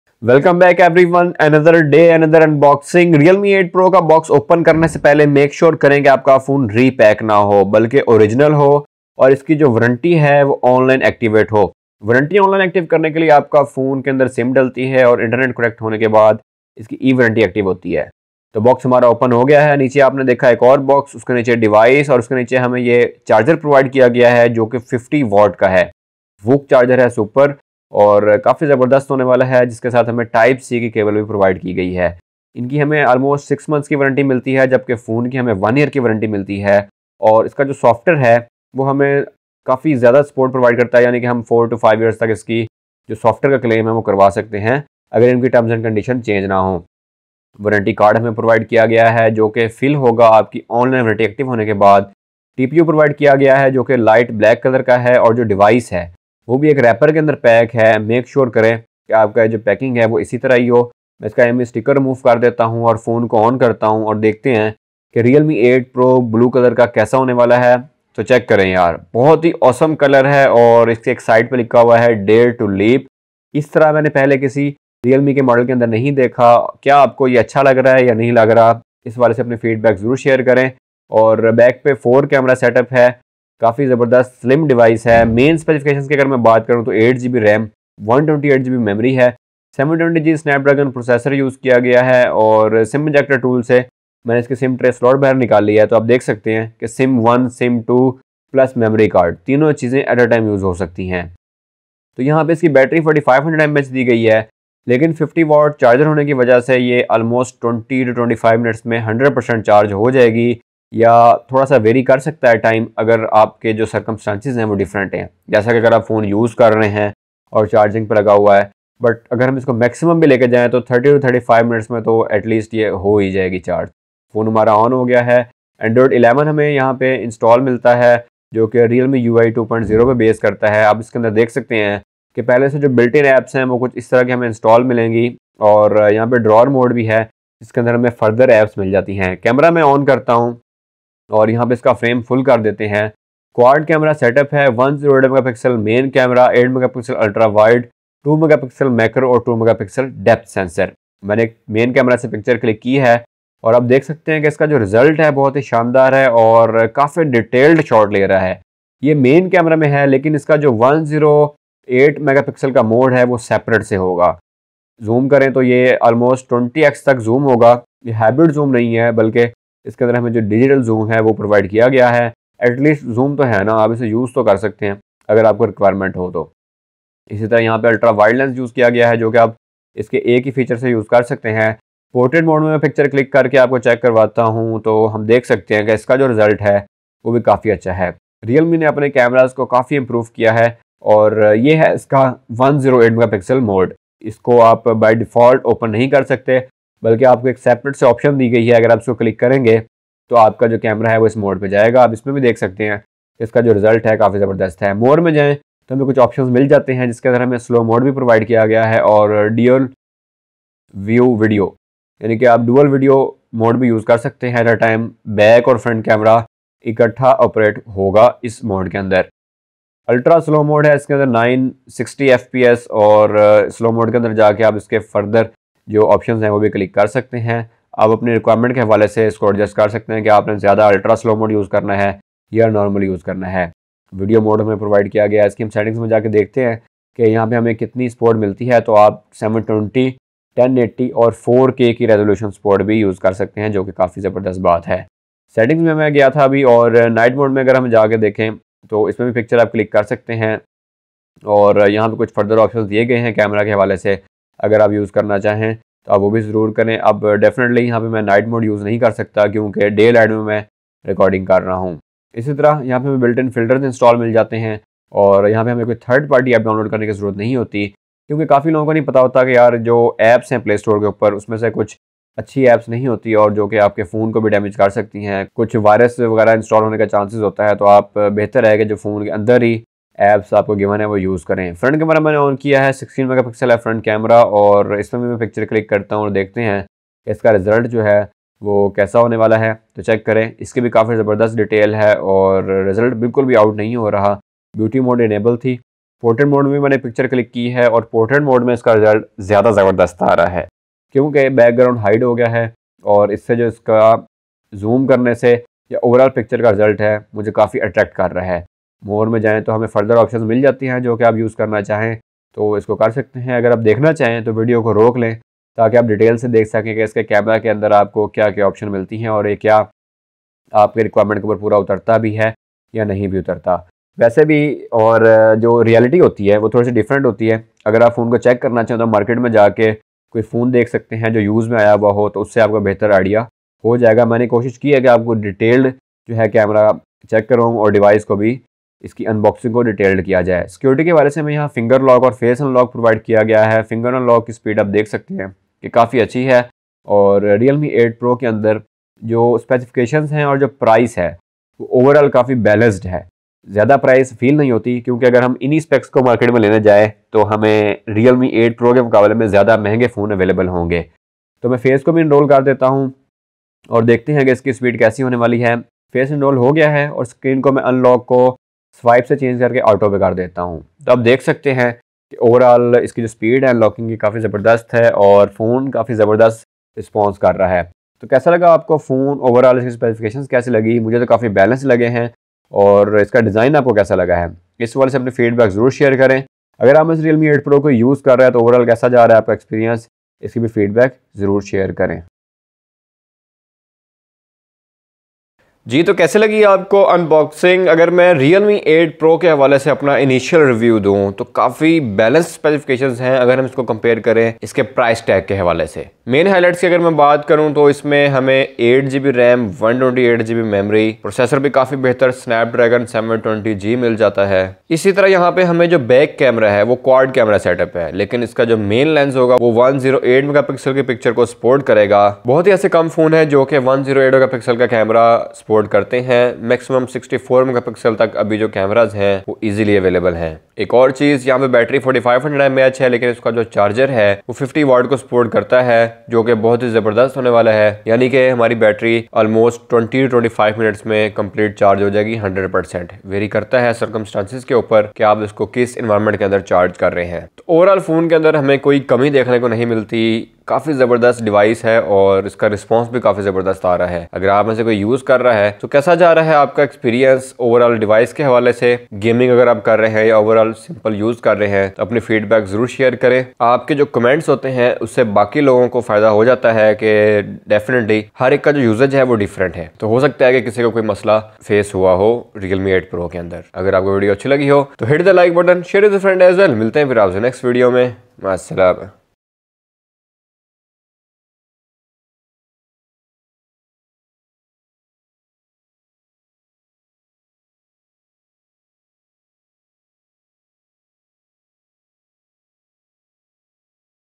realme 8 Pro UNBOXING AND REVIEW - After 7 Days!! (Infinite BLUE)
In this video we will do unboxing of realme 8 Pro. Let's REVIEW REALME 8PRO in detail.

★☆★ THE GURU TALKS ★☆★

♠About♠

[TheGuruTalks:]
TECH | Entrepreneurship | Social